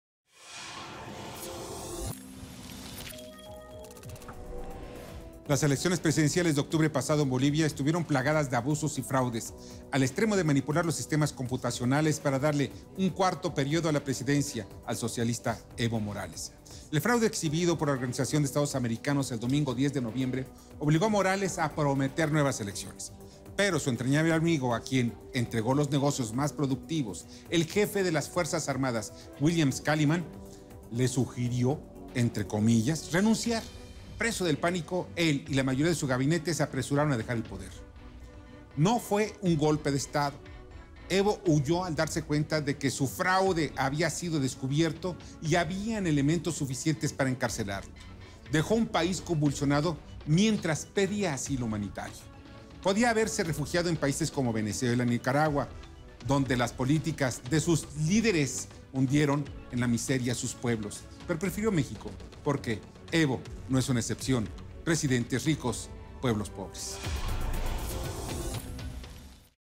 EVO MORALES, ACUSADO DE FRAUDE ELECTORAL
Las elecciones presidenciales de octubre pasado en Bolivia estuvieron plagadas de abusos y fraudes, al extremo de manipular los sistemas computacionales, para darle un cuarto periodo en la Presidencia, al socialista Evo Morales.